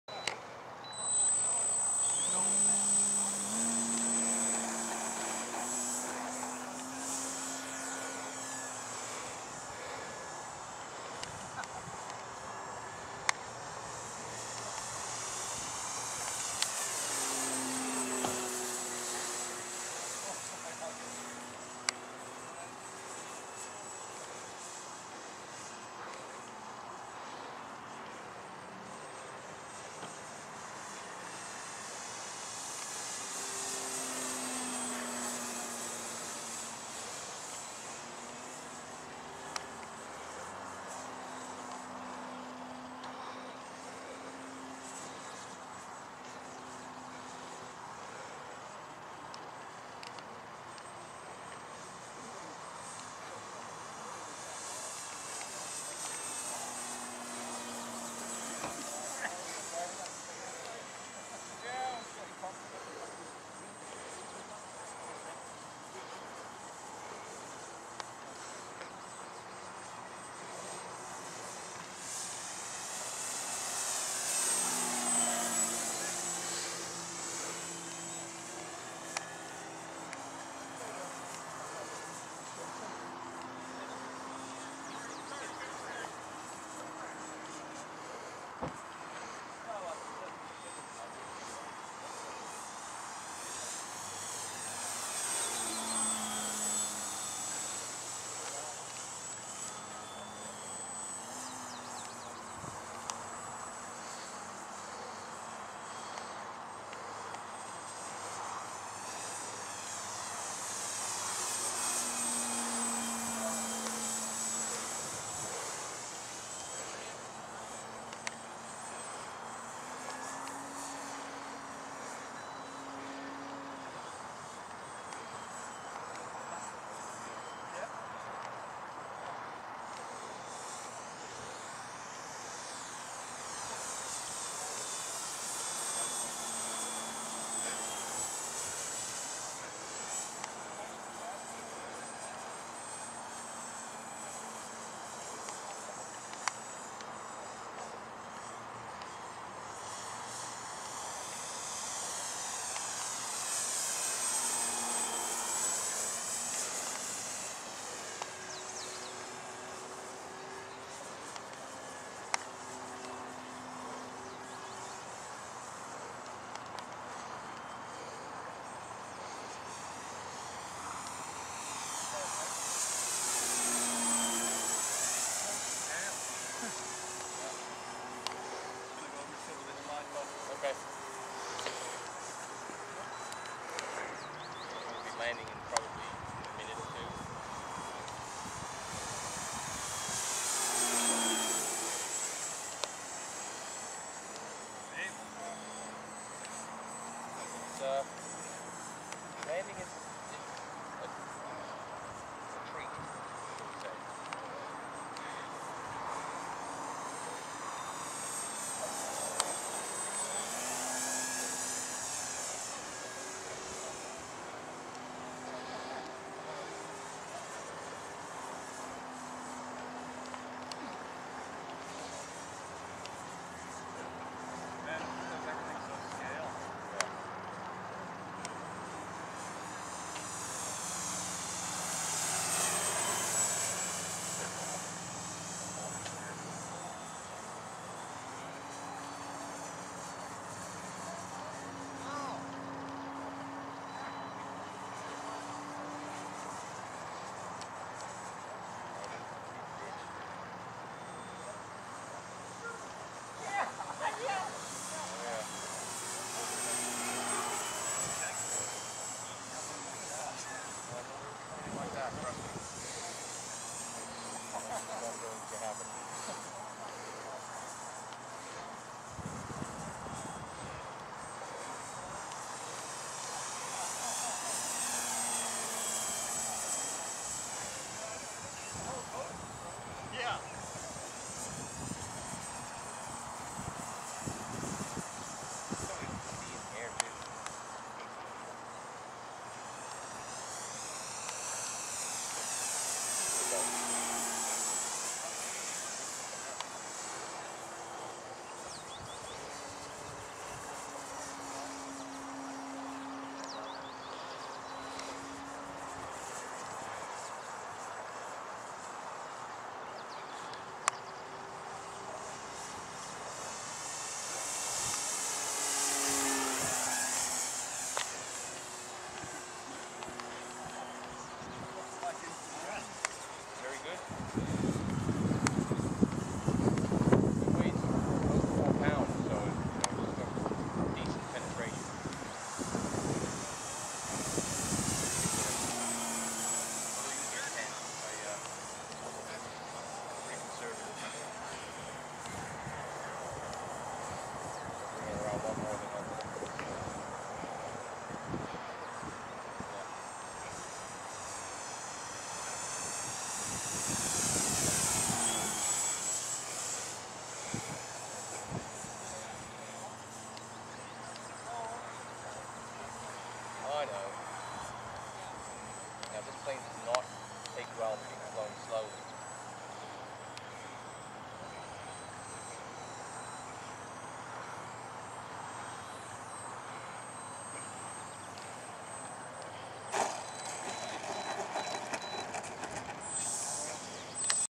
Stearman PT-17 Aug 14 2011
My new plane, 3rd flight. Amazing to fly!